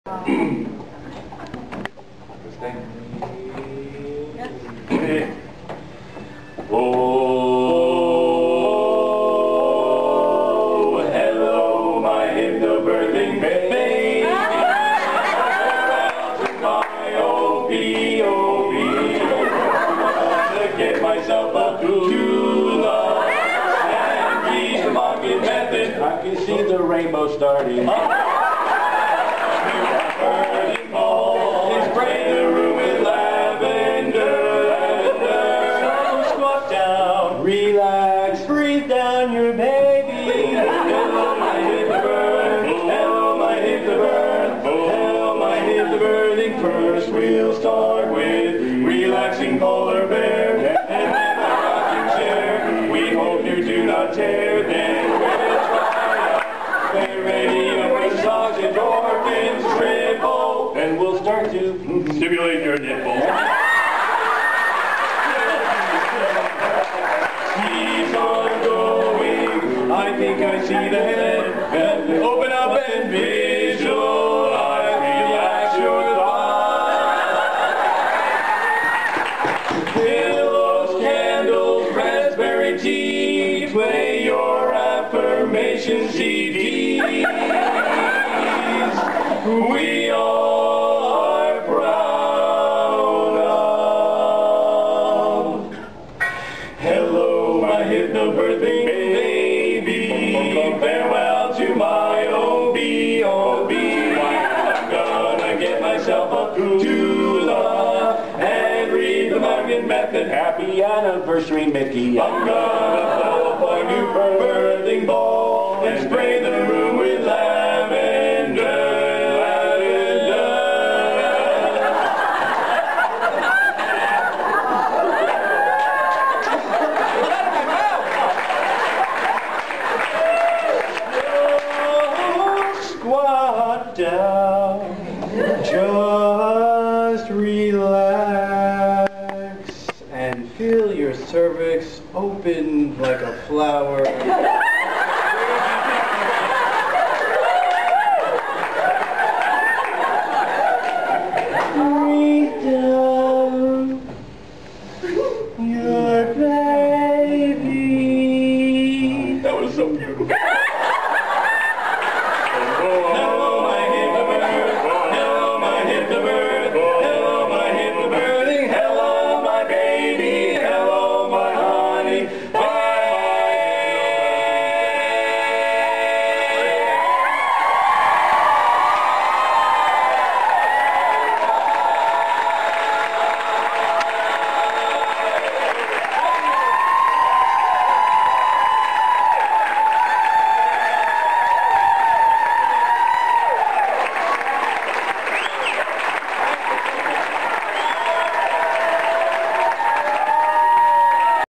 HotShots Sing at the 2009 Hypno Birthing conclave
Singing for the Hypno Birthing conclave for Marie Mongan